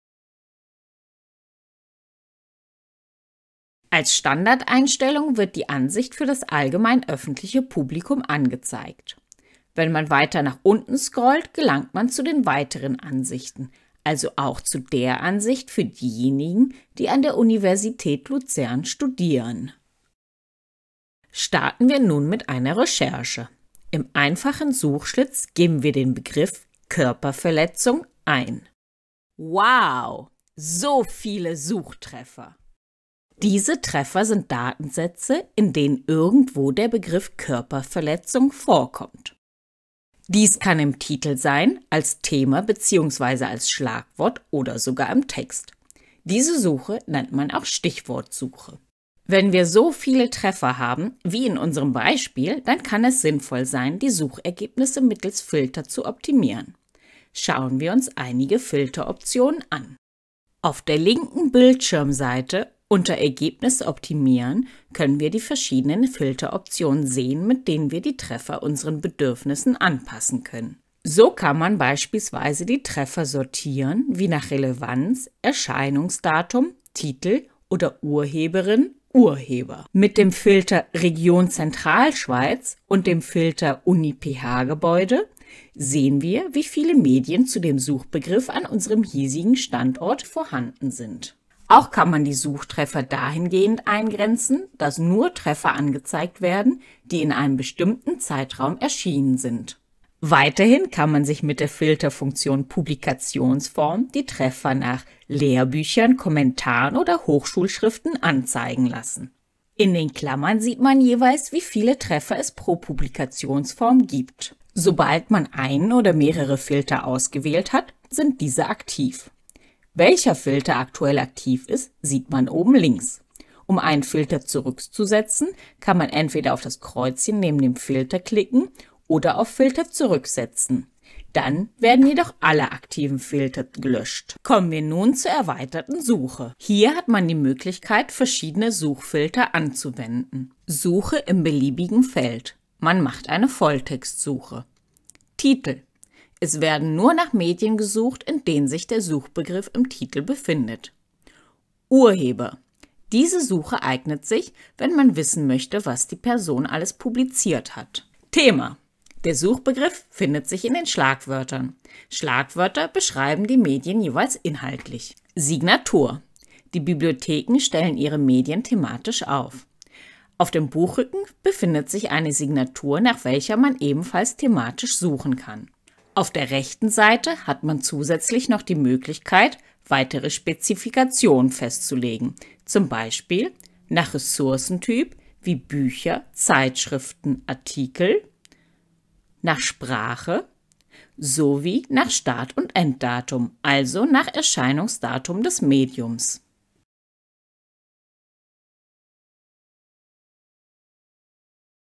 Wie suche ich in swisscovery RZS für Jurist*innen?
In diesem Video zeigen wir Ihnen, wie Sie in swisscovery RZS schnell und einfach nach juristischer Literatur suchen können.